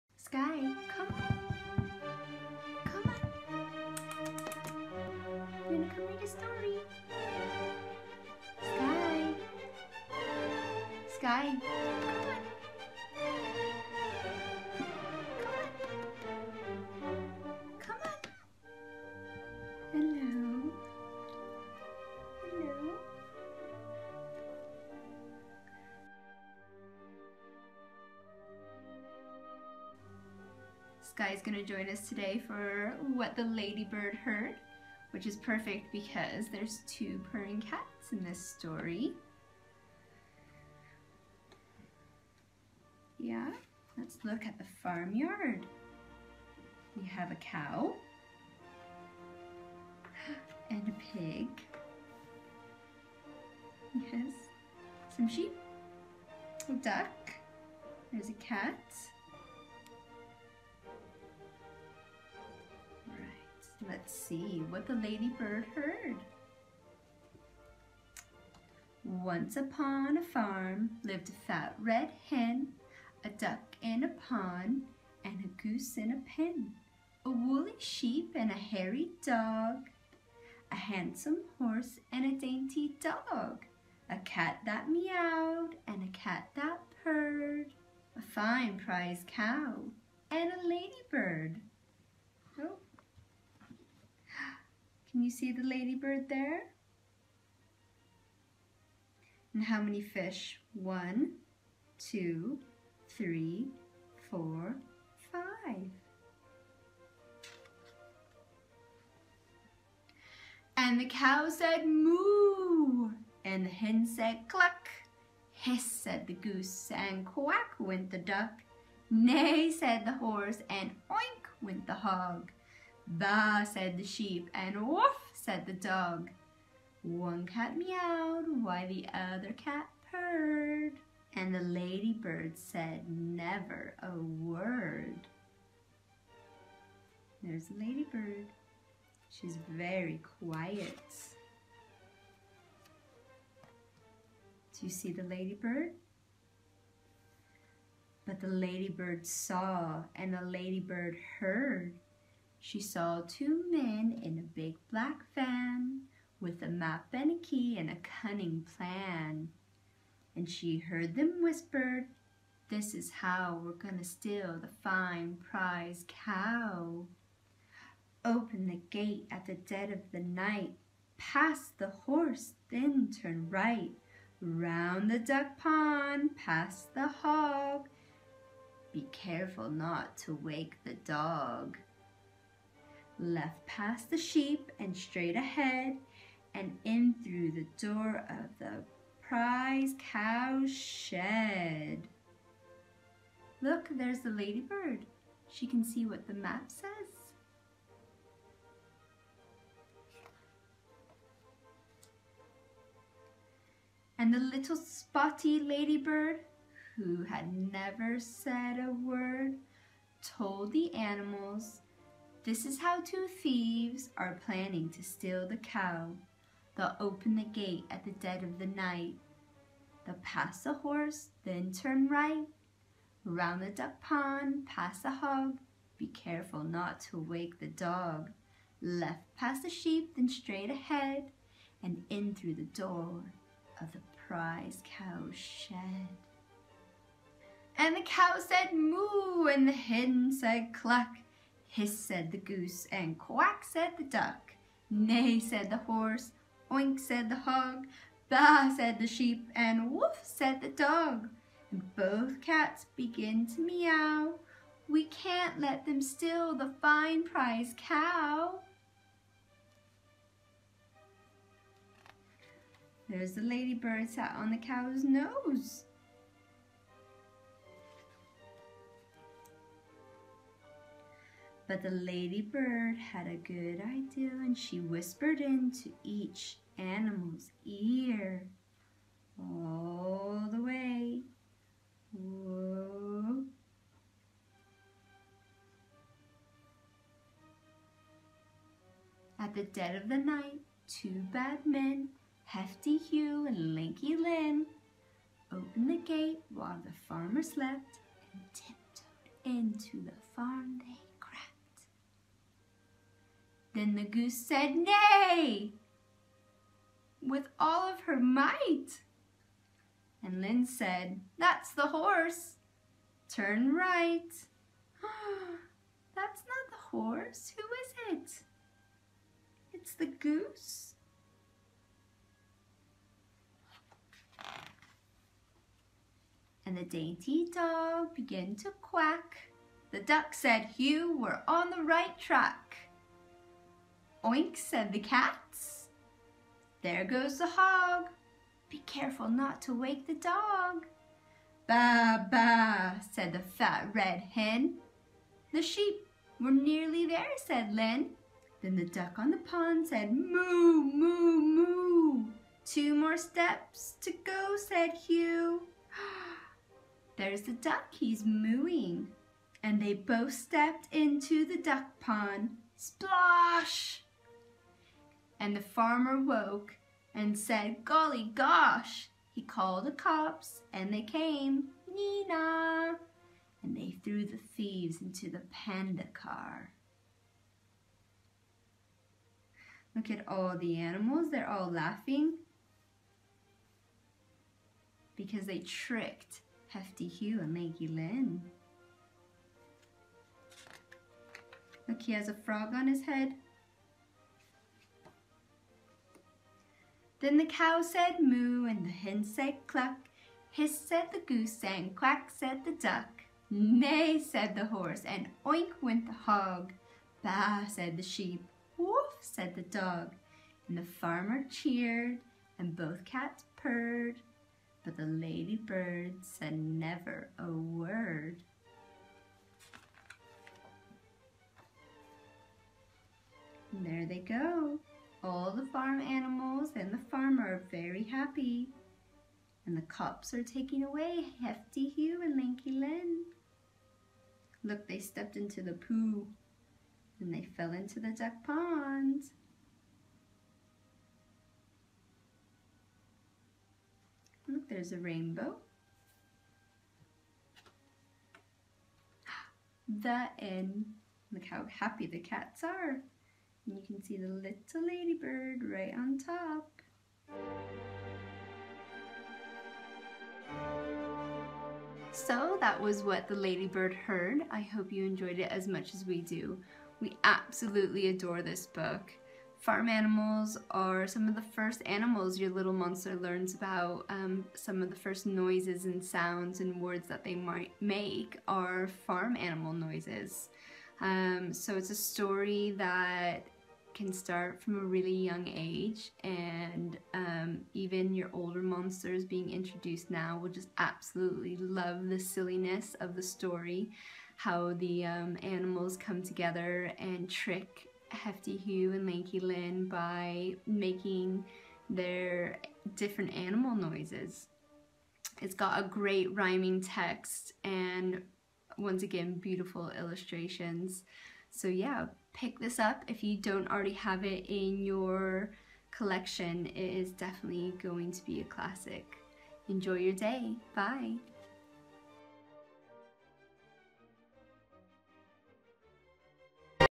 What the Ladybird Heard
All the animals on the farm seem very ordinary, with their MOOing, HISSing, BAAAing and Clucking, until one night, when Hefty Hugh and Lanky Len hatch a plot to steal the fine prize cow. It's now up to them to saves the day, led by one very quite ladybird. We love how Julia has taken ordinary farm animals and made them extraordinary, a great twist on a classic farm yard! What the Ladybird Heard is such a wonderful book for all stages of language development- from the very little monster repeating the animal sounds, to the older little monsters, who can engage with the enchanting flowing rhythmic text.